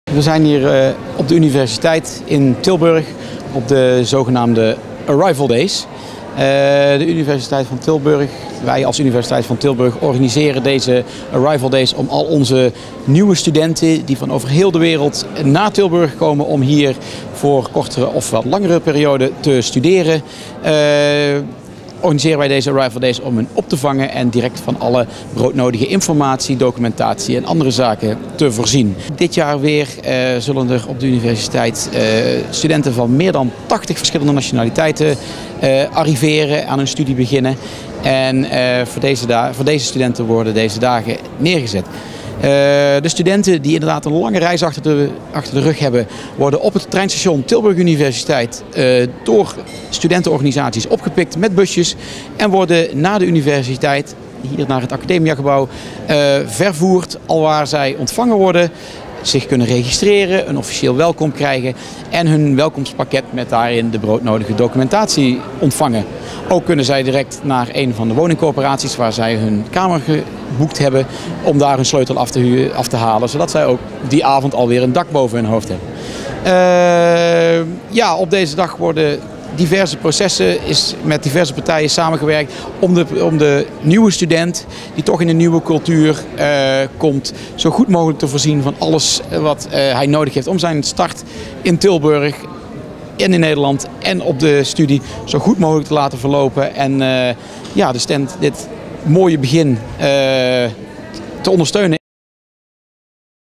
Arrivals Days Tilburg University 2012
The Arrival days are the start of the introduction week 2012 for new international students at Tilburg University.